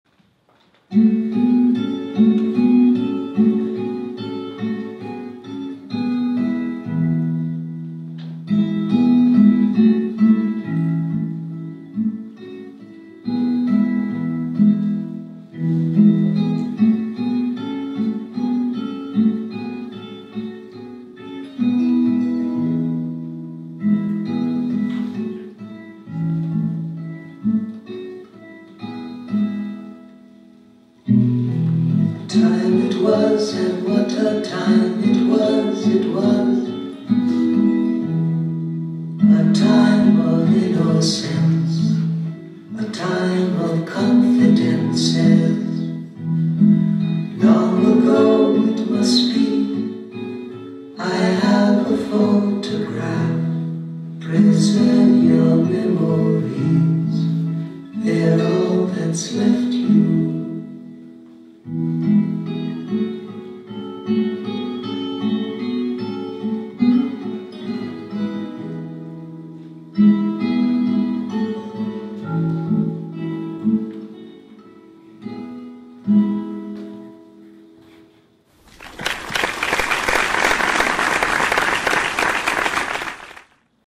Bookends (2016) - Choreography by Kayley Thompson
Dancer: Priscilla Vasquez
Choreography by Kayley Thompson
Performed at Phoenix Center for the Arts on April 8, 2017.